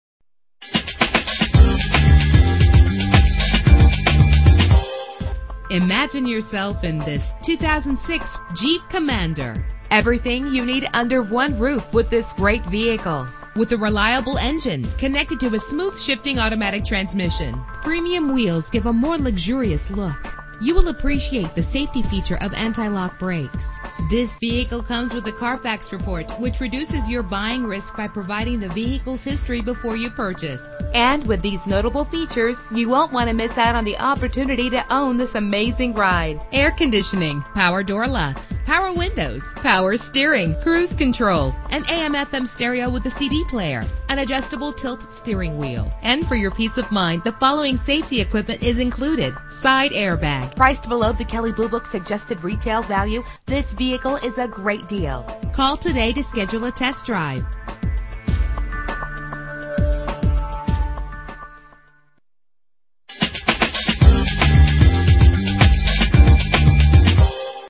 2006 Jeep Commander - Downey CA
Year: 2006
Make: Jeep
Model: Commander
Trim: Limited Sport Utility 4D
Engine: 8 cylinder 4.7 Liter
Transmission:
Color: Beige
Mileage: 64239
Address:
9655 E. Firestone Blvd